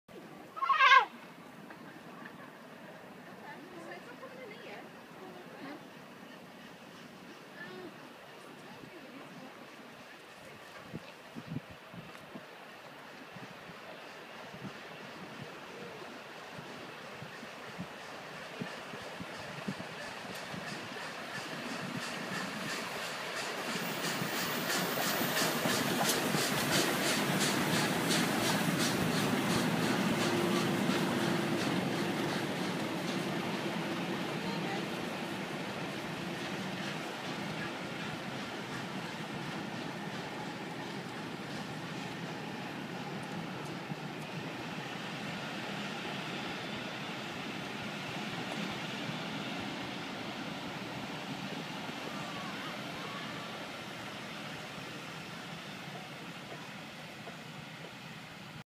46233 duchess of Sutherland and class 37516
46233 duchess of sutherland and class 37516 at York on Scarborough flyer on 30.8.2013